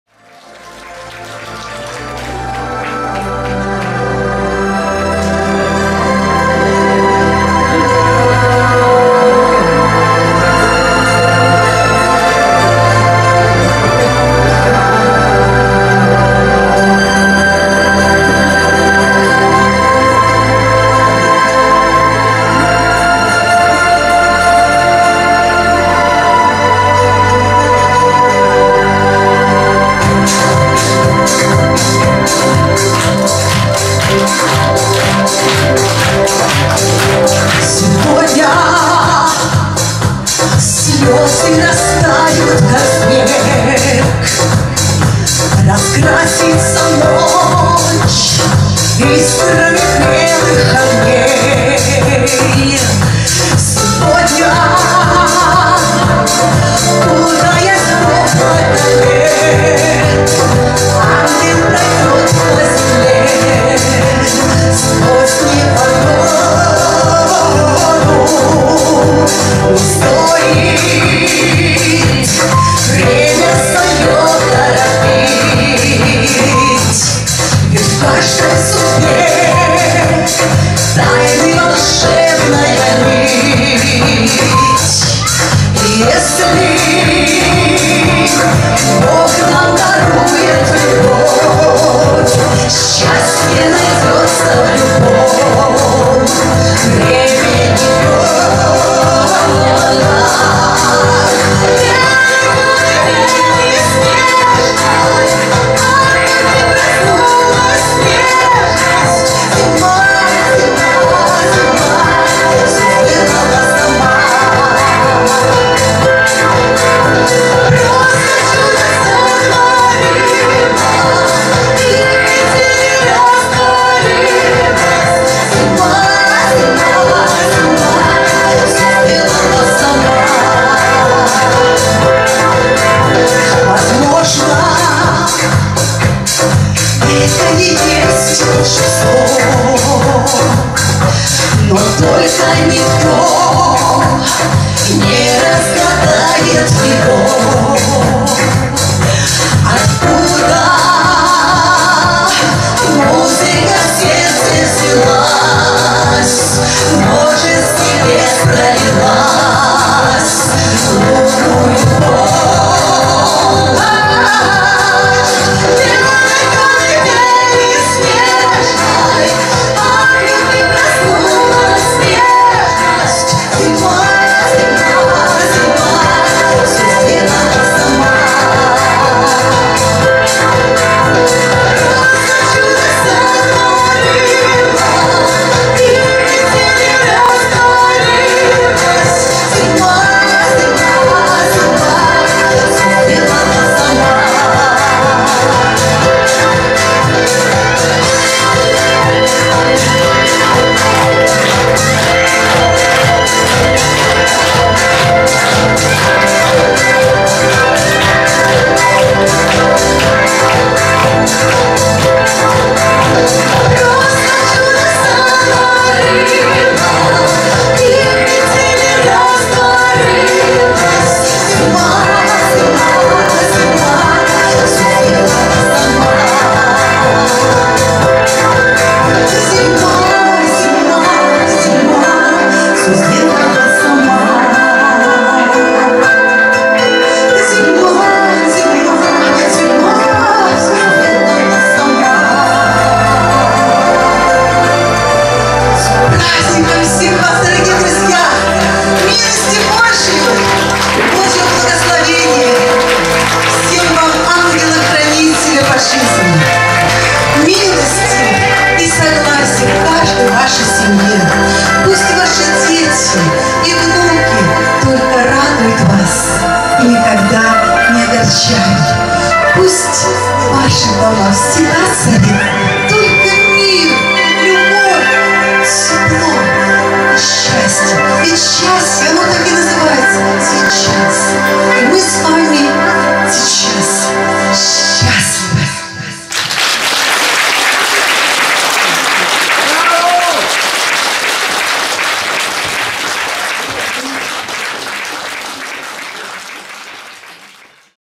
Татьяна КРАВЧЕНКО
(город Солигалич)
в концертной программе
"РОЖДЕСТВЕНСКИЙ КОНЦЕРТ-2014"
ЦД "Шанс", село Введенское.